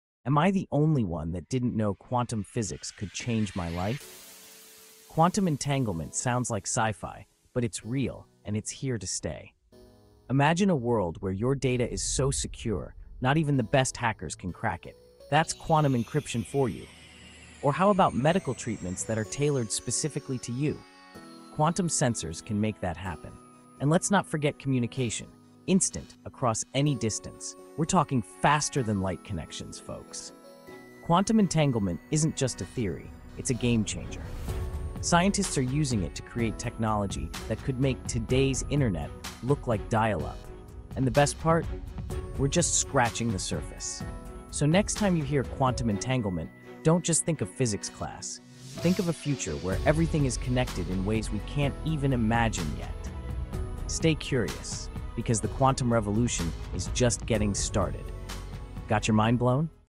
Quantum Entanglement CHANGES EVERYTHING in Real Life!
Quantum Entanglement CHANGES EVERYTHING in Real Life! Did you know that the phenomenon of quantum entanglement has the potential to revolutionize the way we live our daily lives? From secure communication networks to advanced medical treatments, the implications of entanglement are far-reaching and mind-boggling. In this video, we'll explore the incredible ways in which quantum entanglement is changing everything, from the way we think about reality to the way we interact with each other. Get ready to have your mind blown by the endless possibilities that entanglement has to offer!